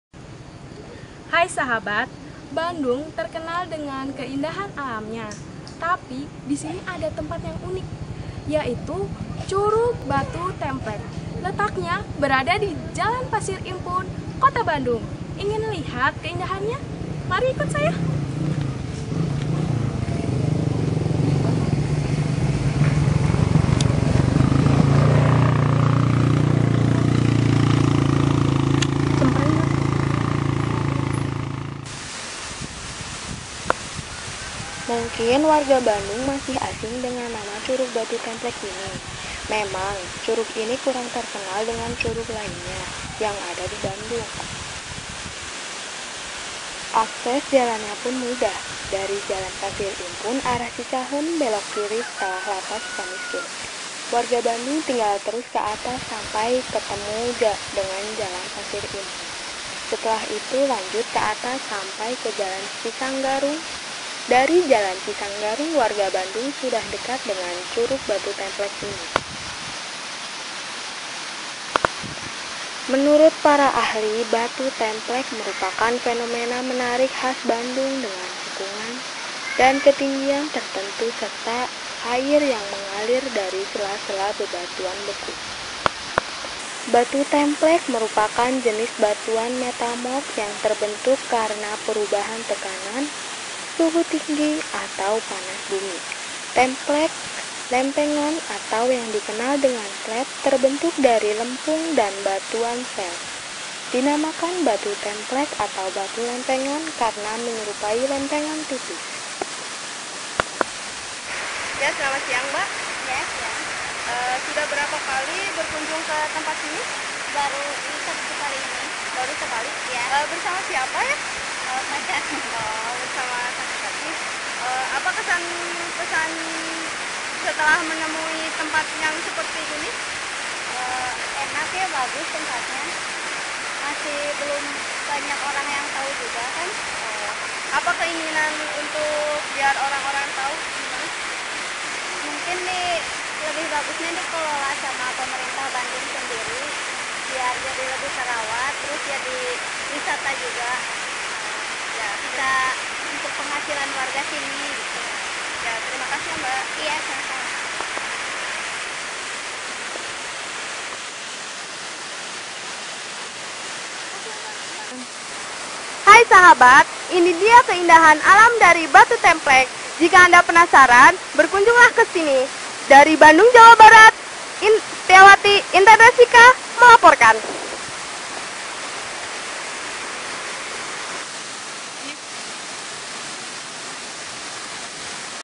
ADVENTURE: Wisata Air Curug Batu Templek
Video liputan bareng adik tingkat pas masa kuliah. Curugnya adem.. belum banyak yang tau. Recomended buat niis dan refresh otak.